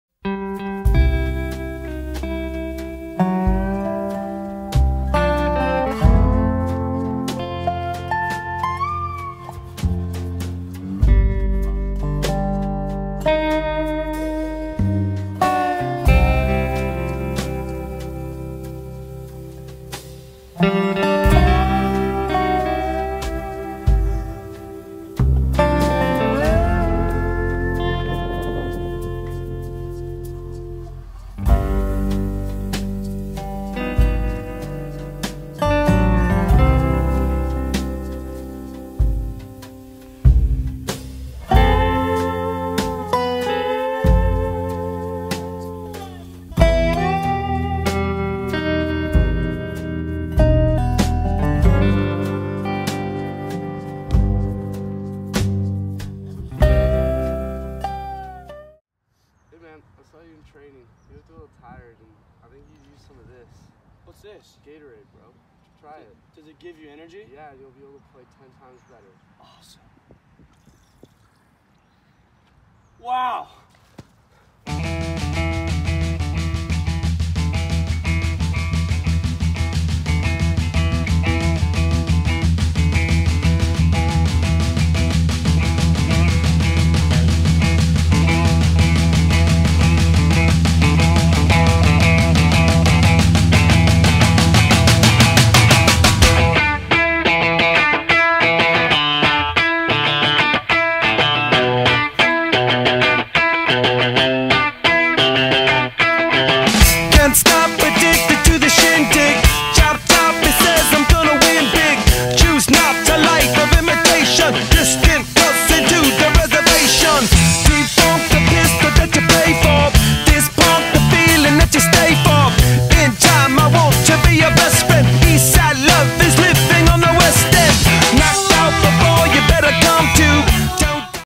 Derek's Jan Term Gatorade Commercial
Commercial for Gatorade created for my Storytelling January Term course.